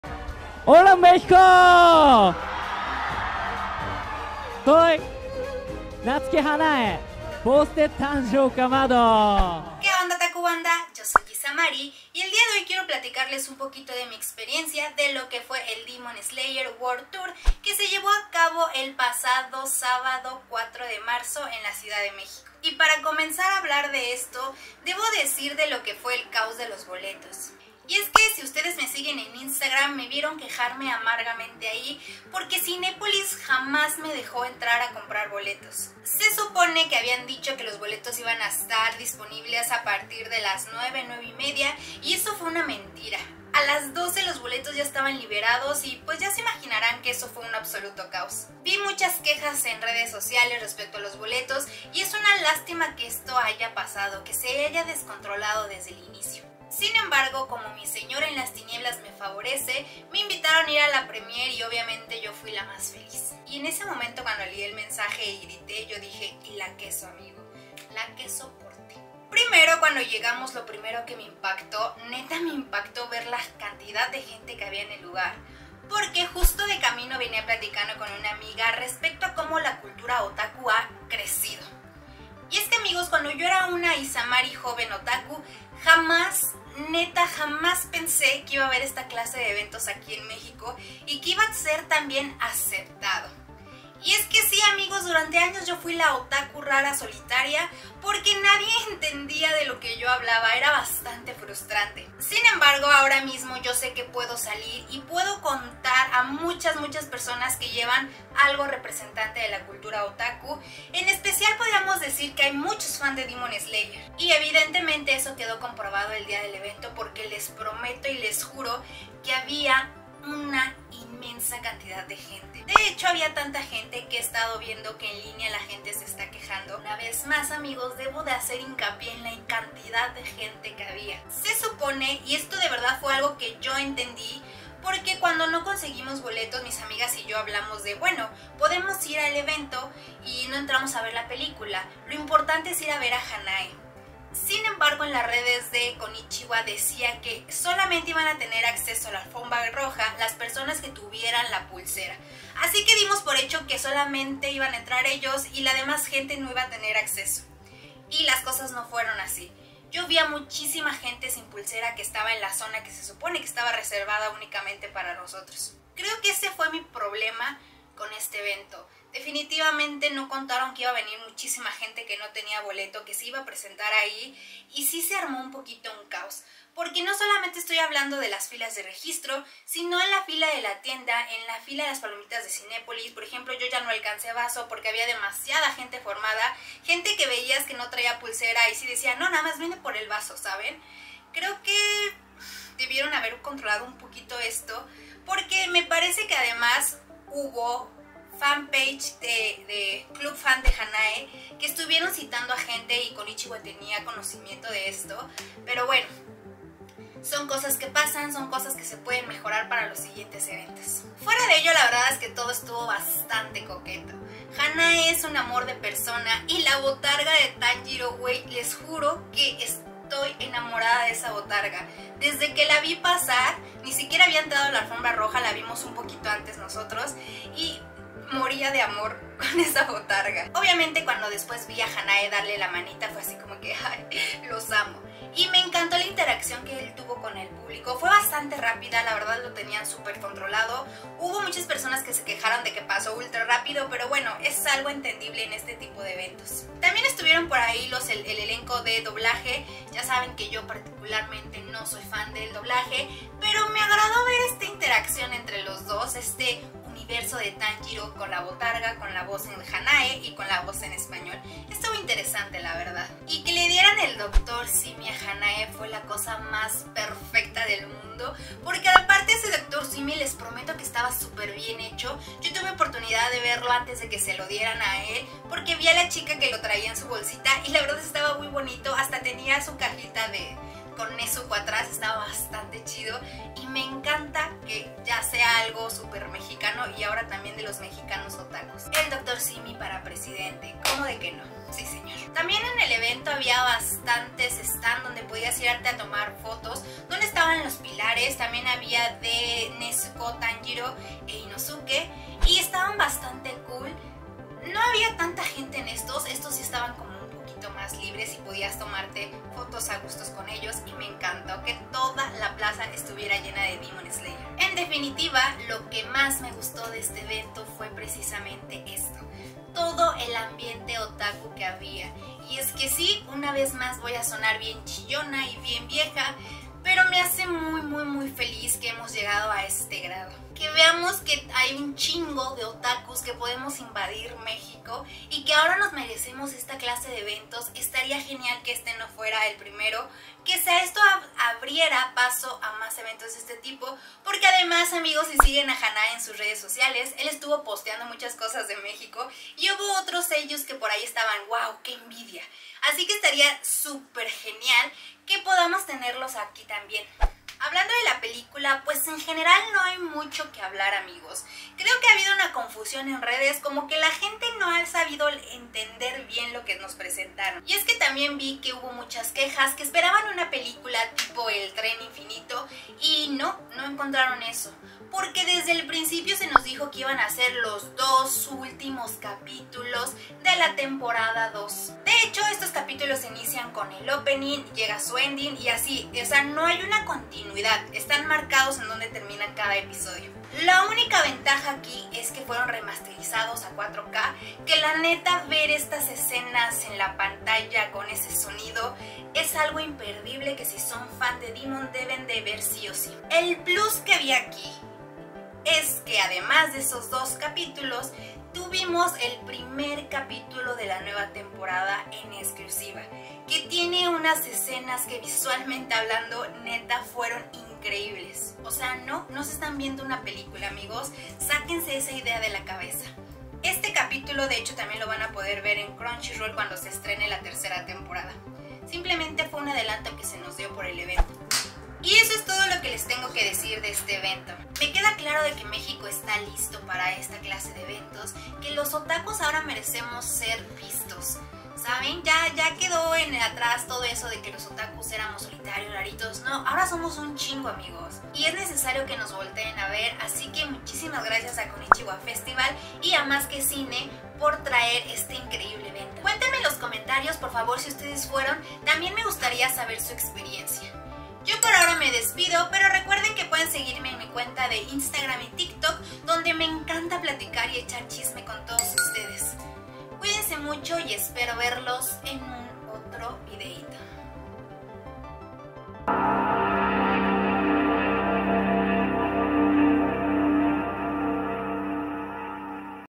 DEMON SLAYER WORLD TOUR CDMX + Natsuki Hanae en México
¿Qué onda, otaku banda?
(⁠〃ﾟ⁠3ﾟ⁠〃⁠)

En este vídeo les quiero contar mi experiencia en el Demon Slayer World Tour que se realizó el pasado sábado, en donde centenares de otakus celebraron la llegada de la tercera temporada de Demon Slayer y la visita de  Natsuki Hanae

💕 ¡VAMOS A SEGUIRNOS! 💕